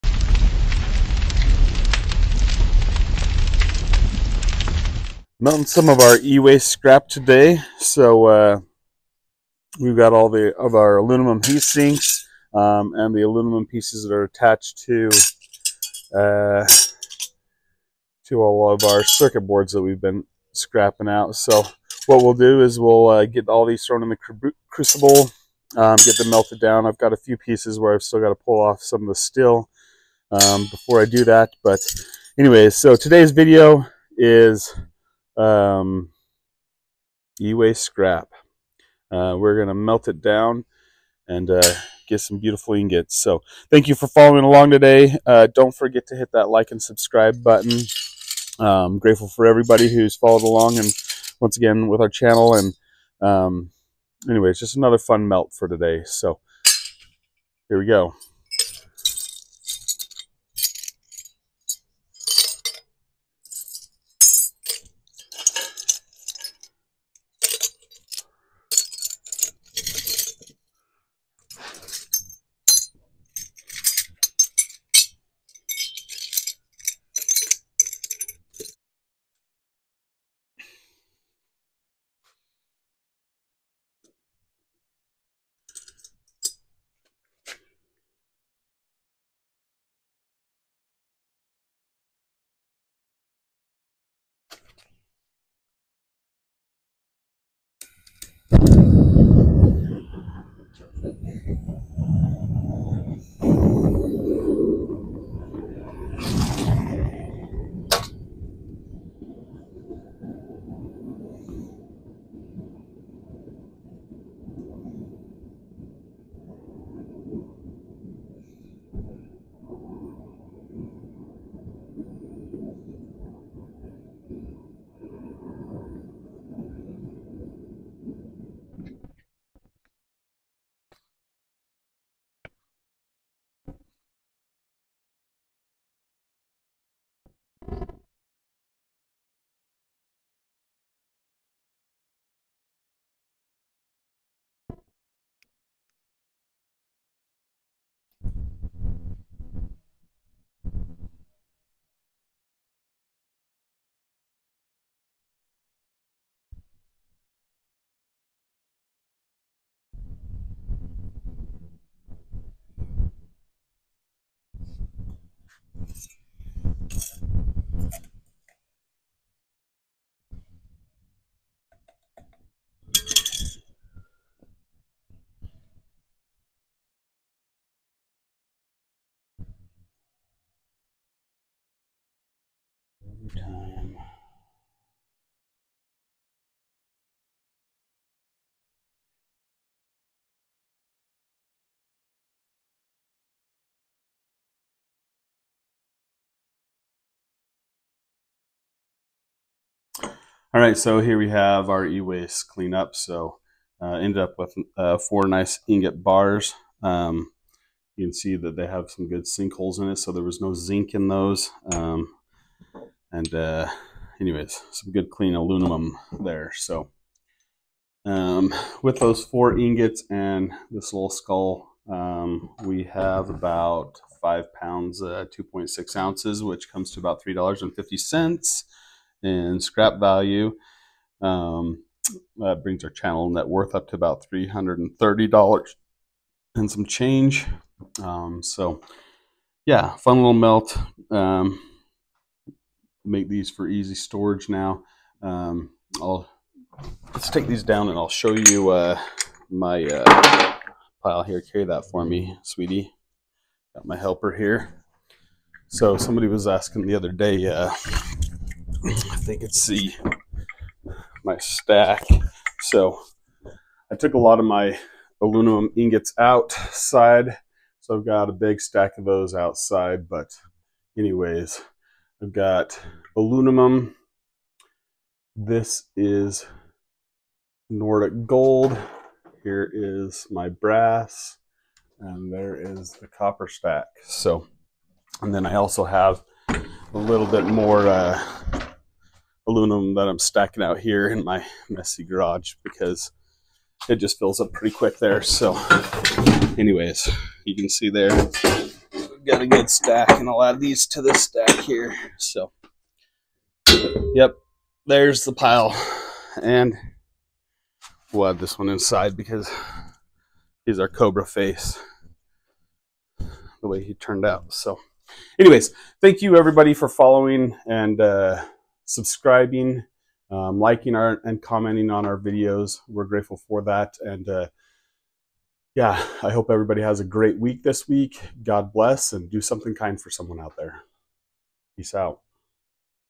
Ewaste Scrap | Aluminum Melt
Scrapped out a couple dozen computers and different electronics. We took all the ewaste scrap and melting it down. Then poured the molten metal into ingots.

Making Bullions out of old scrap metal. Turning old chunks of precious metal into bullion that can be melted and molded into unlimited potential.

29